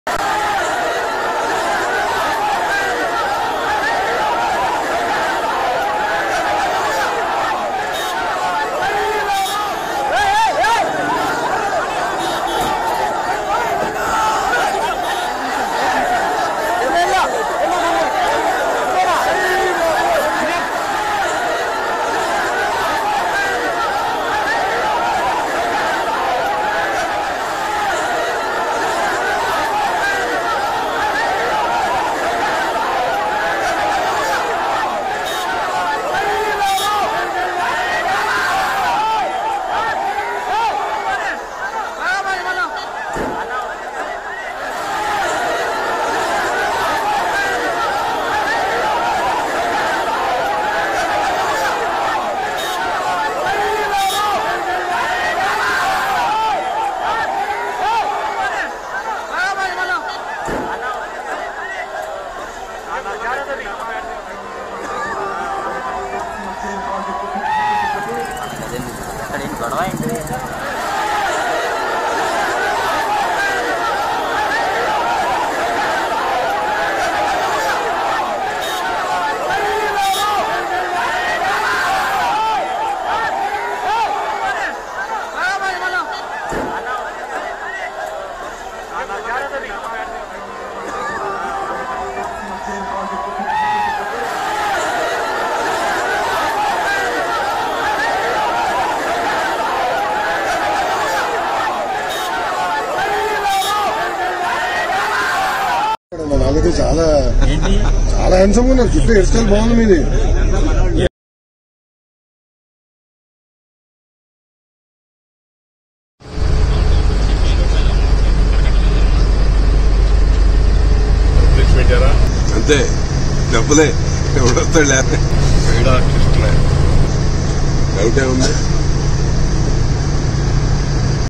డబ్బులివ్వకపోతే ఎవరొస్తారు Farmers Protesting Aganist Chandrababu In Amaravathi | Social TV Telugu
Farmers Protesting Aganist Chandrababu Naidu Over Graphic Buildings In Amaravathi | Social TV Telugu